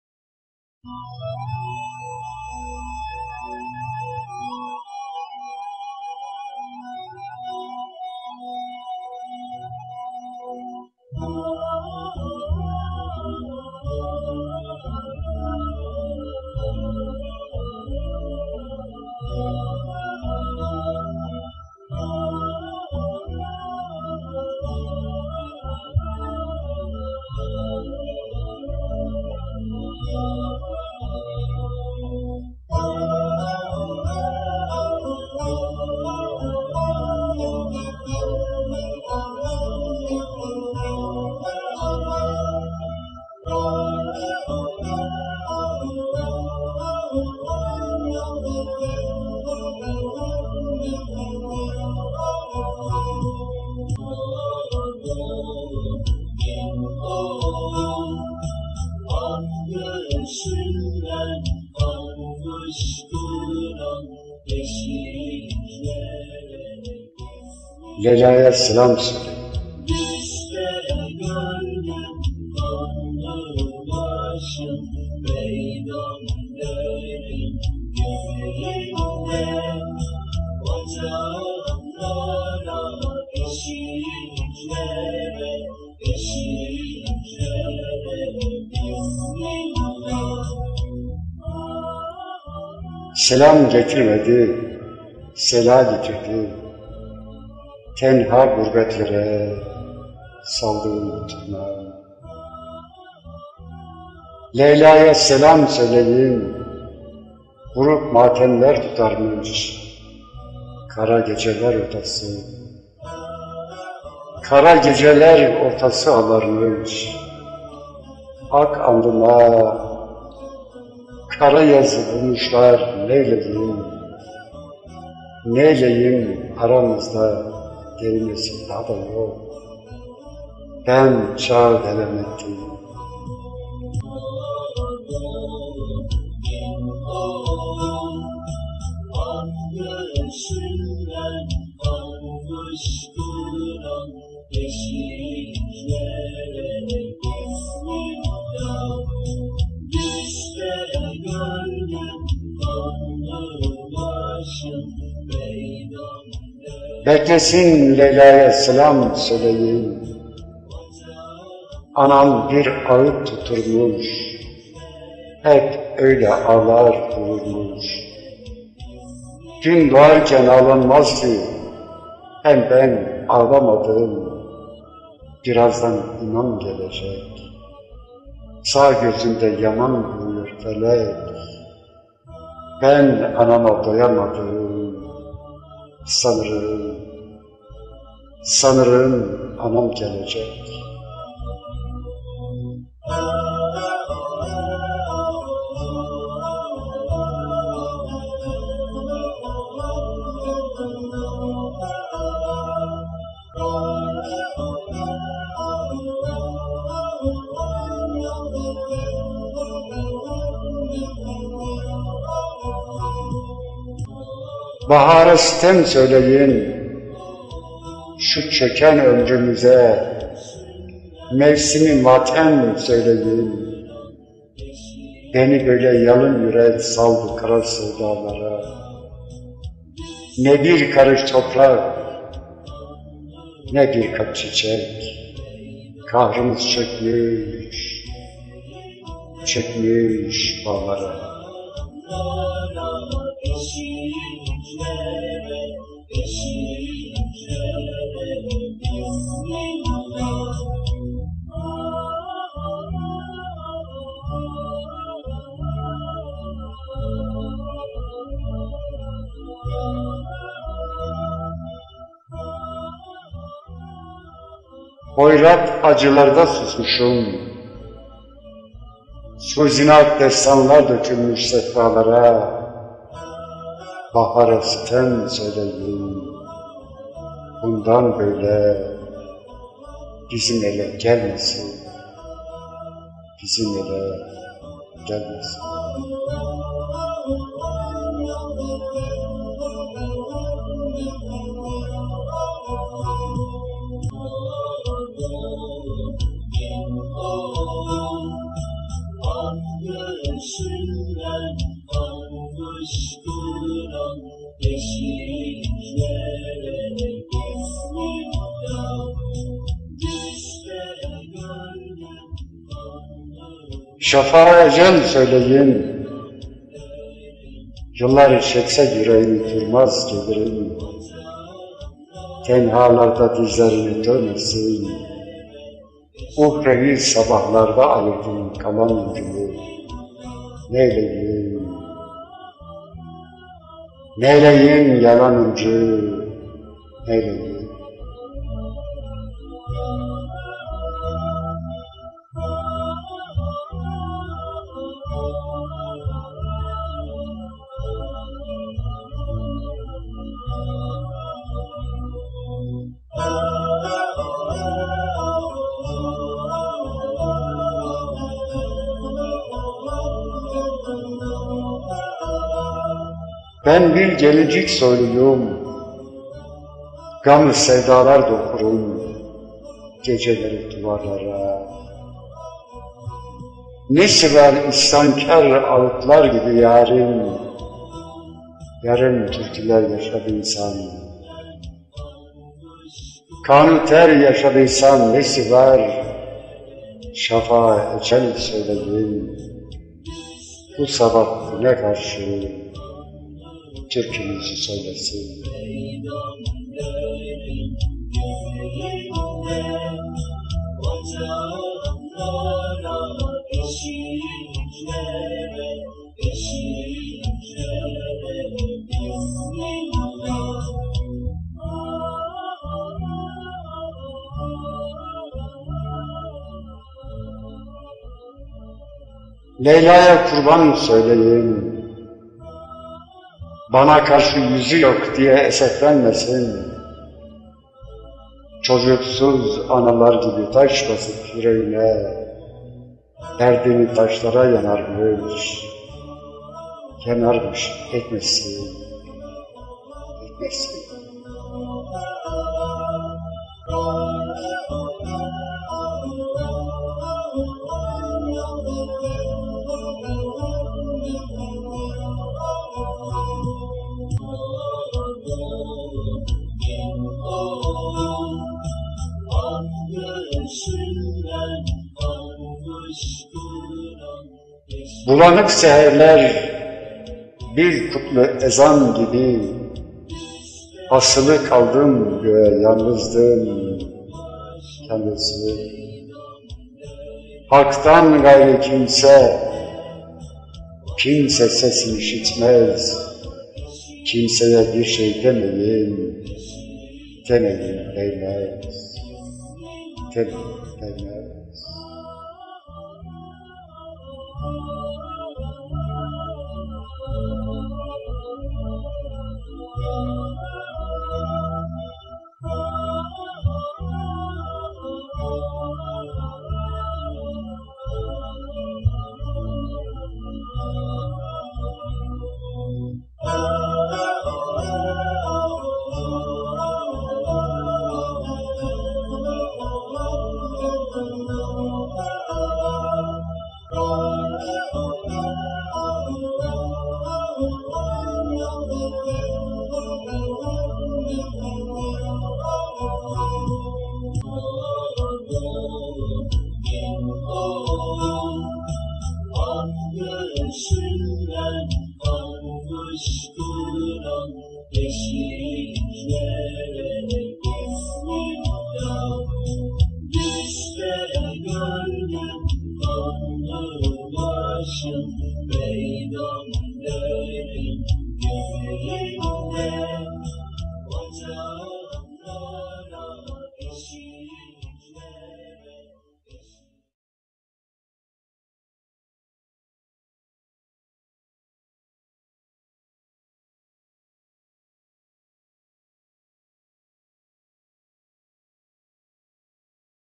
LEYLA'YA SELAM SÖYLEYİN SÜLEYMAN AĞA BAYDİLİ VİDEOSU
LEYLA'YA SELAM SÖYLEYİN

Selam getirmedi selâ getirdi
Tenha gurbetlere saldığım Tunam
Leyla'ya Selam söyleyin
Buruk matemler tutarmış kara geceler ortası
Kara geceler ortası ağlarmış.
Ak alnıma kara yazı vurmuşlar neyleyim.
Neyleyim aramızda delinesi dağ'da yok
Ben çağı delemekteyim

Beklesin Leyla'ya selam söyleyin
Anam bir ağıt tutturmuş,hep öyle ağlar dururmuş.
Gün doğarken ağlanmaz ki,
Hem ben ağlamadım..
Birazdan imam gelecek,
Sağ gözümde yaman vuruyor Felek
Ben anama doyamadım,
Sanırım anam gelecek

Bahara sitem söyleyin
Şu çöken ömrümüze mevsimi matem söyleyin
Beni böyle yalın yürek saldı kara sevdalara
Ne bir karış toprak,ne birkaç çiçek
Kahrımız çökmüş bağlara..

Hoyrat acılarda susmuşum,
Süz-i nak destanlar dökülmüş sehpalara
Bahara sitem söyleyin
Bundan böyle bizim ele gelmesin..

Şafağa ecel söyleyin.
Yollar çekse yüreğimi durmaz gelirim
Tenhalarda dizlerini dövmesin.
Uhrevi sabahlarda ayırdım kalan ömrü
Neyleyim neyleyim yalan ömrü..!!
Neyleyim..!!

Ben bir gelincik soyluyyum.
Gamlı sevdalar dokurum,
Geceleri duvarlara..
Nesi var isyankar ağıtlar gibi yarım türküler yaşadıysam
Kan-u ter yaşadıysam nesi ver.
Şafağa ecel söyleyin
Her sabah güne karşı türkümüzü söylesin..

Leyla'ya kurban söyleyin.
Bana karşı yüzü yok diye eseflenmesin.
Çocuksuz analar gibi taş basıp yüreğine,
Derdini taşlara yanarmış..ETMESİN..!!

Bulanık seherler bir kutlu ezan gibi
Asılı kaldım göğe yanlızdım yanlızım..
HAK'tan gayrı kimse sesim işitmez
Kimseye birşey demeyin değmez

Şiir:
Süleyman Ağa Baydili
Yorum & Animasyon
Durak Yiğit